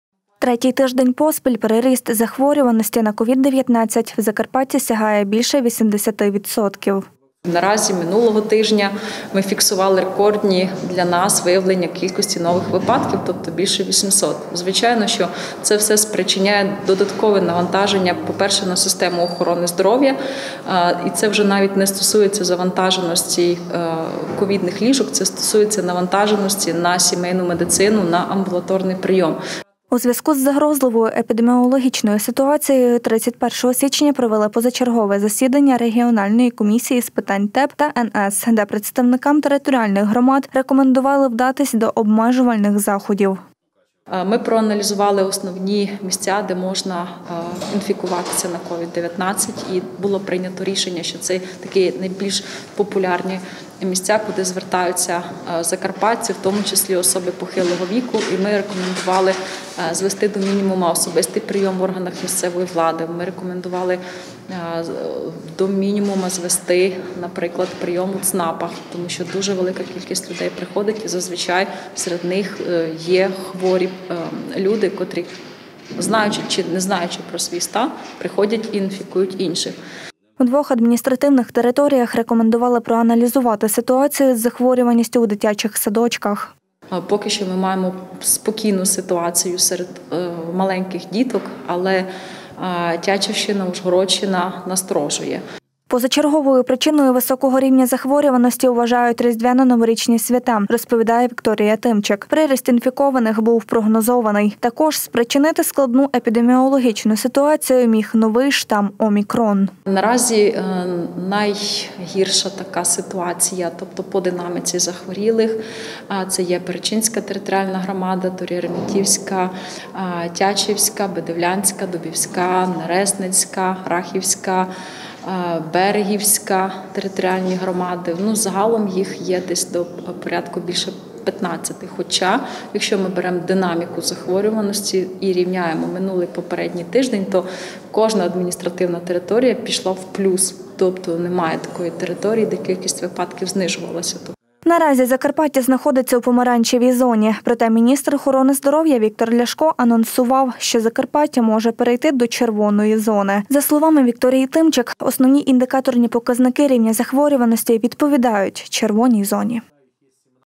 Прийом громадян. Переходять на онлайн.
У зв’язку з загрозливою епідеміологічною ситуацією відбулося позачергове засідання регіональної комісії з техногенно-екологічної безпеки та надзвичайних ситуацій. Третій тиждень приріст захворюваності збільшився на більше ніж 80%. Представникам територіальних громад рекомендували перейти на дистанційну роботу та приймати громадян в режимі онлайн. Третій тиждень поспіль приріст захворюваності на COVID-19 в Закарпатті сягає більше 80%.У зв’язку з загрозливою епідеміологічною ситуацією 31 січня провели позачергове засідання регіональної комісії з питань ТЕБ та НС, де представникам територіальних громад рекомендували вдатись до обмежувальних заходів.В двох адміністративних територіях рекомендували проаналізувати ситуацію з захворюваністю в дитячих садочках.Першочерговою причиною високого рівня захворюваності вважають різдвяно-новорічні свята, розповідає Вікторія Тимчик. Приріст інфікованих був прогнозований. Також, спричинити складну епідеміологічну ситуацію міг новий штам «Омікрон».Наразі Закарпаття знаходиться в «помаранчевій» зоні, проте, міністр охорони здоров’я Віктор Ляшко анонсував, що Закарпаття може перейти до «червоної» зони. За словами Вікторії Тимчик, основні індикаторні показники рівня захворюваності відповідають «червоній» зоні.